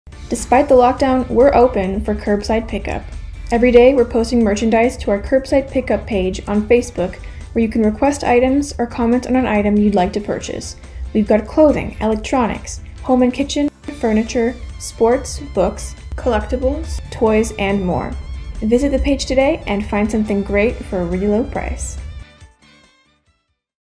We are open for Curbside Pickup!
Curbside pickup is available from 10:30 to 4:30, Monday to Saturday, until in-person shopping can resume.
to request or claim items.